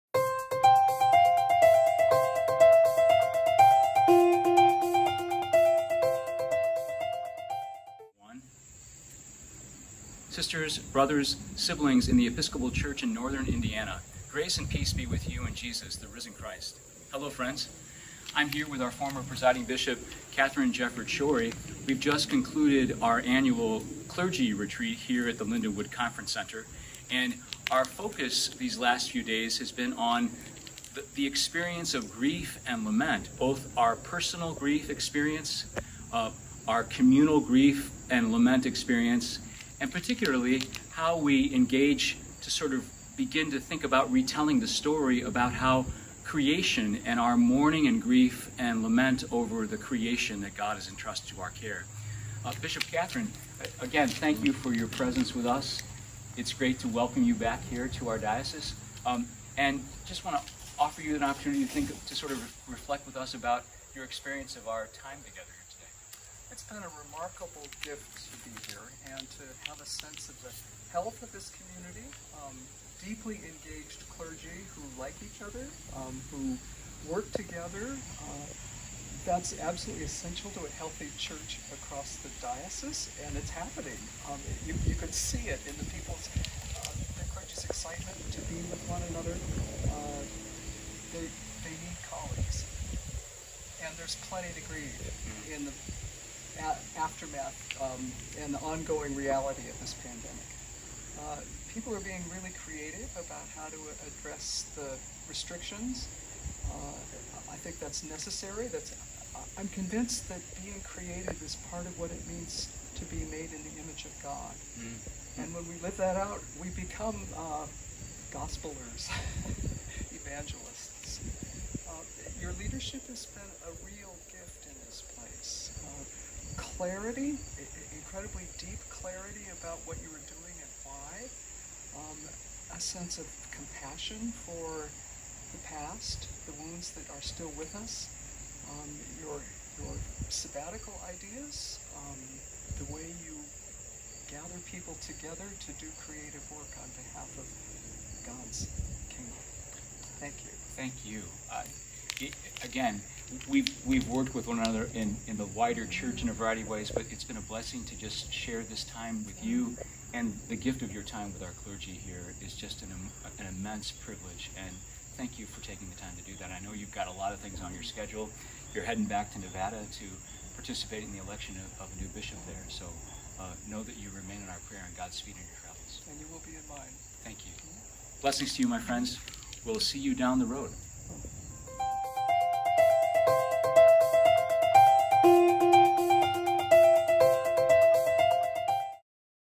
Fall Clergy Retreat 2021 Recap
Bishop Doug Sparks and Former Presiding Bishop Katharine Jefferts Schori reflect on the Fall Clergy Retreat held October 4-6, 2021 at Lindenwood Retreat and Conference Center.